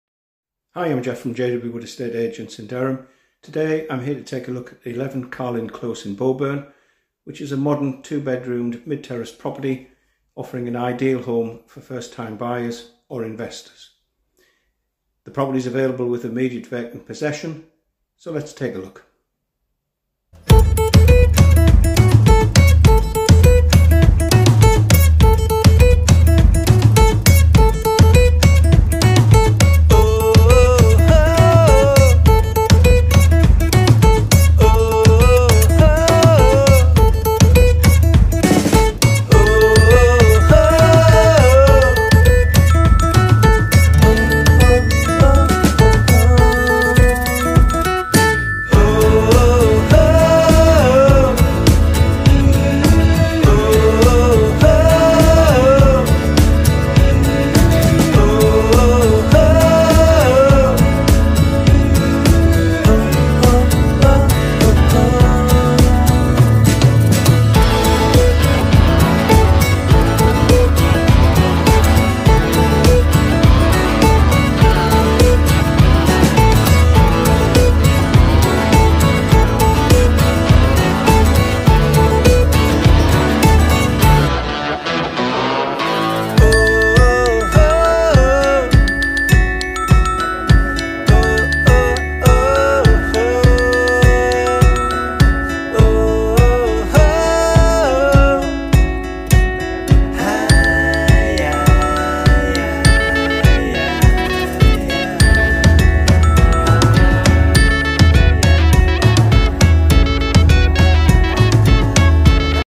2 BED HOUSE FOR SALE. 11 Carlin Close, Bowburn, Durham, DH6 5FA.
Available with the benefit of immediate vacant possession, we offer for sale this modern mid terraced property with gas fired central heating and double glazing.

Ideally suited to first time buyers or investors, it provides well presented accommodation briefly comprising: entrance hall with cloakroom/wc, lounge having French doors to the rear garden and a well fitted kitchen with built in appliances.  On the first floor there are two bedrooms and a bathroom/wc.  It has easy to maintain gardens to the front and rear and a parking space to the front.

Carlin Close is a pleasant cul-de-sac of modern properties situated within easy reach of everyday shops etc which are available within Bowburn itself.  There are more comprehensive shopping and recreational facilities and amenities available within Durham City Centre which lies approximately 3 miles distant.  Bowburn is also well placed for commuting purposes being adjacent to the A(177) Highway which offers access to Durham City and Teesside and close to the A1(M) Motorway Interchange which provides good road links to both North and South and is close by to the Bowburn Amazon Warehouse.